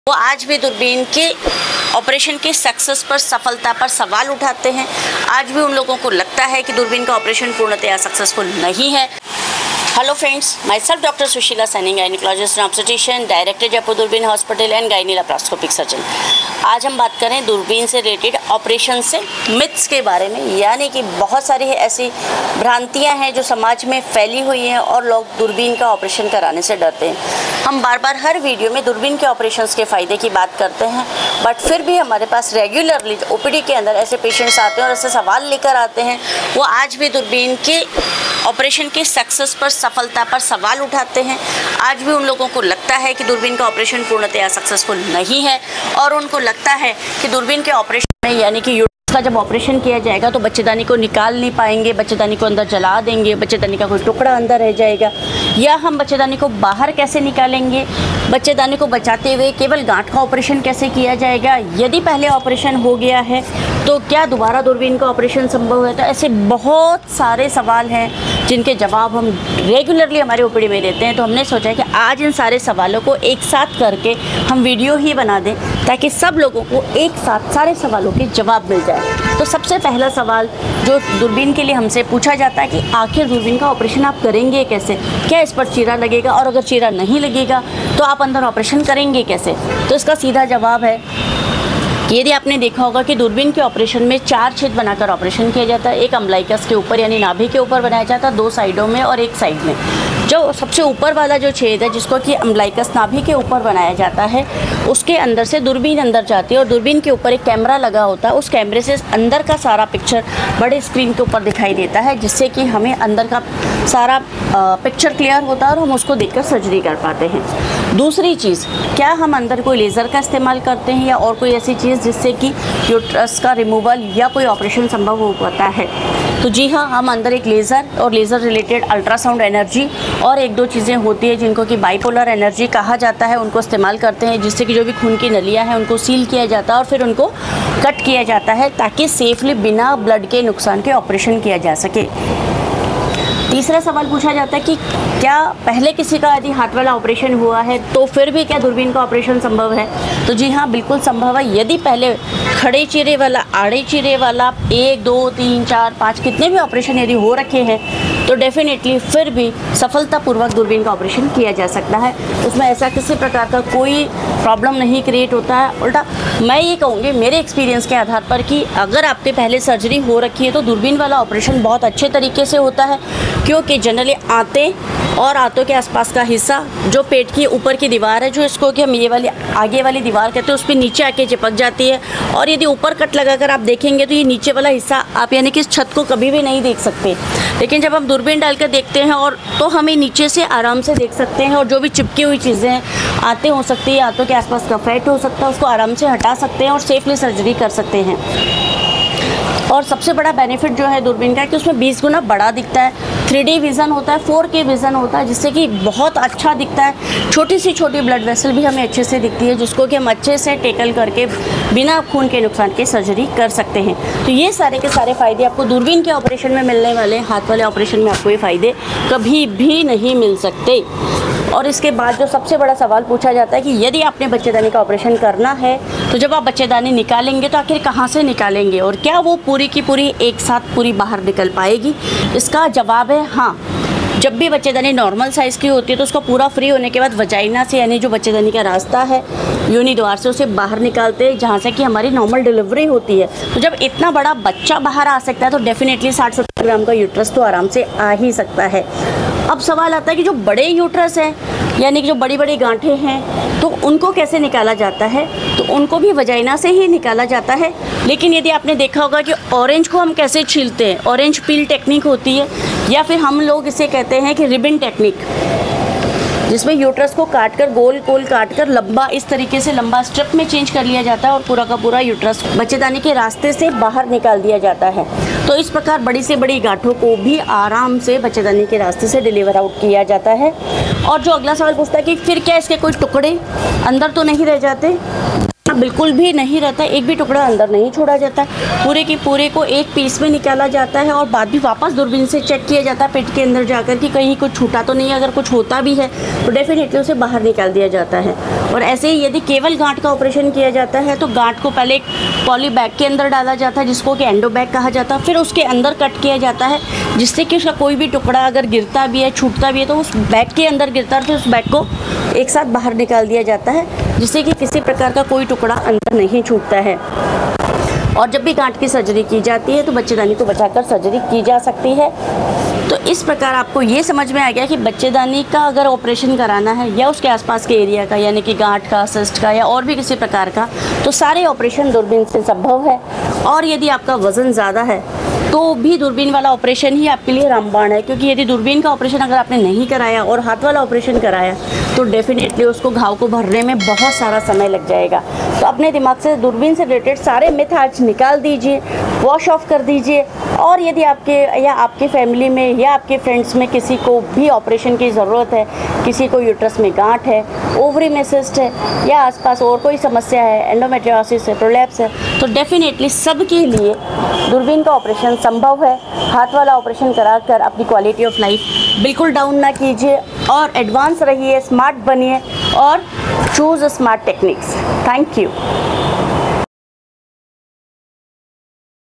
दूरबीन Operation Ke Myths
क्या आप दूरबीन सर्जरी (लेप्रोस्कोपिक सर्जरी) करने से कतराते हैं? कई लोगों का मानना है कि ये ऑपरेशन दर्दनाक हैं और शरीर पर निशान छोड़ देते हैं। @JaipurDoorbeenHospital  के @drsushila-saini  दूरबीन ऑपरेशन के मिथकों को इस वीडियो में समझाते हैं। जानें दूरबीन सर्जरी के लाभ, क्या यह हर किसी के लिए उपयुक्त है, और ऑपरेशन के बाद क्या ध्यान रखना चाहिए। अधिक जानकारी के लिए वीडियो देखें।